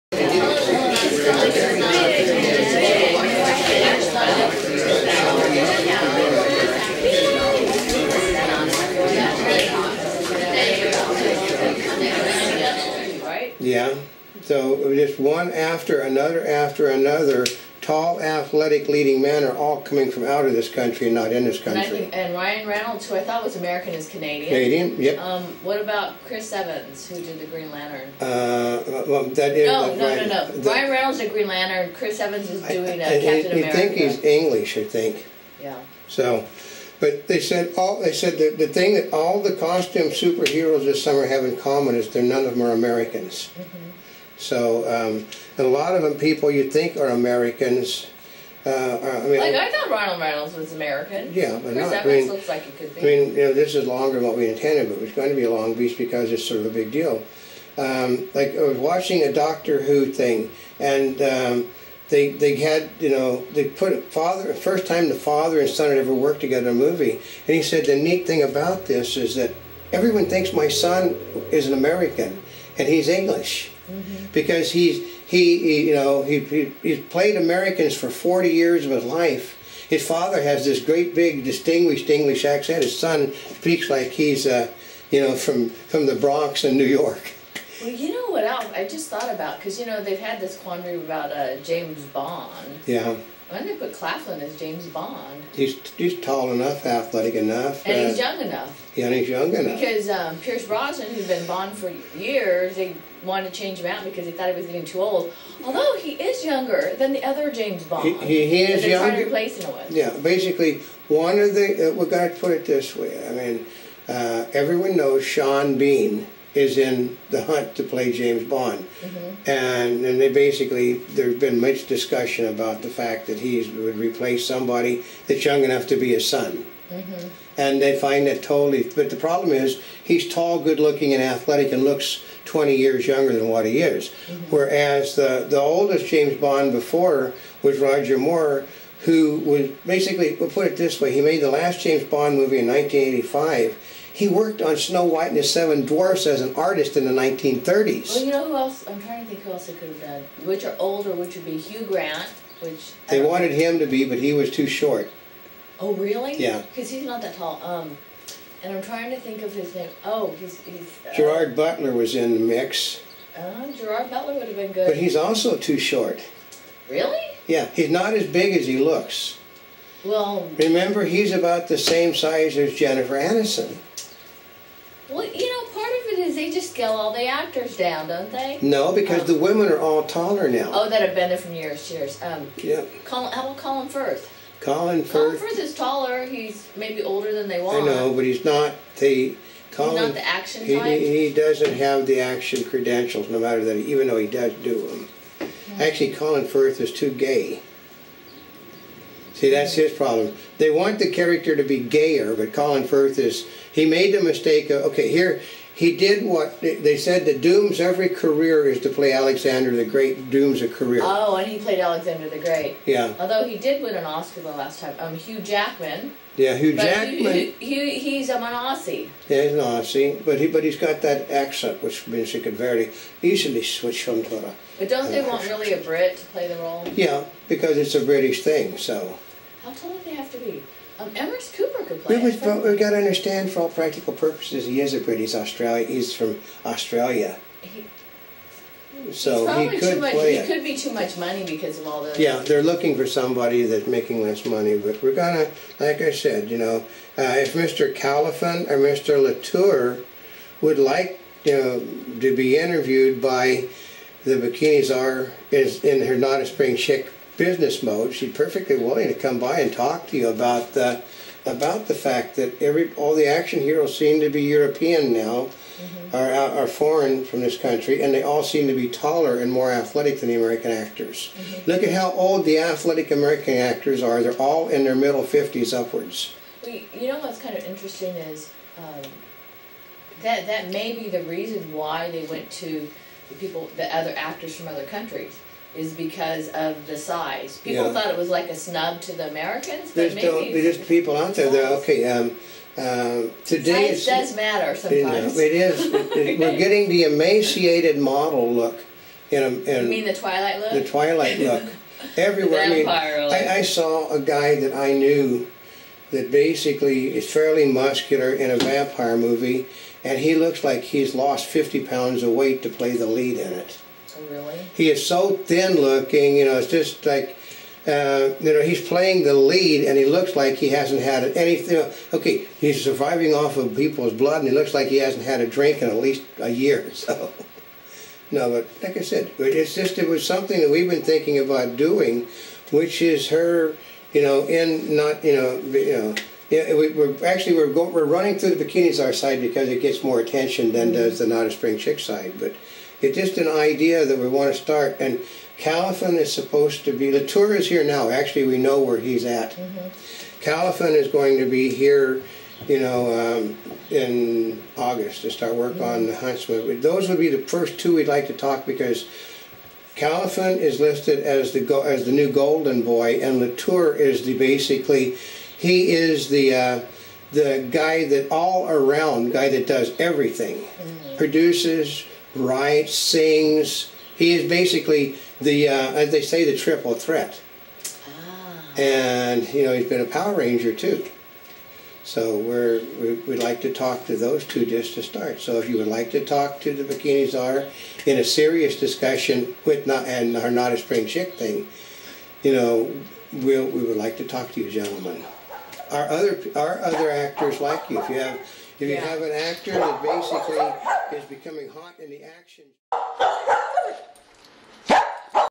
Size does matter when it comes to being a super hero part 2 in 3D
The Bikini Czar and Oldecam for Newsvideoweb with part 2 on why so many foreign actors like Sam Claflin and David de Lautour end up playing leads in American action movies and television, in anaglyph 3D, She is looking forward to doing interviews with these foreign actors to find out why they are getting the leads over American actors.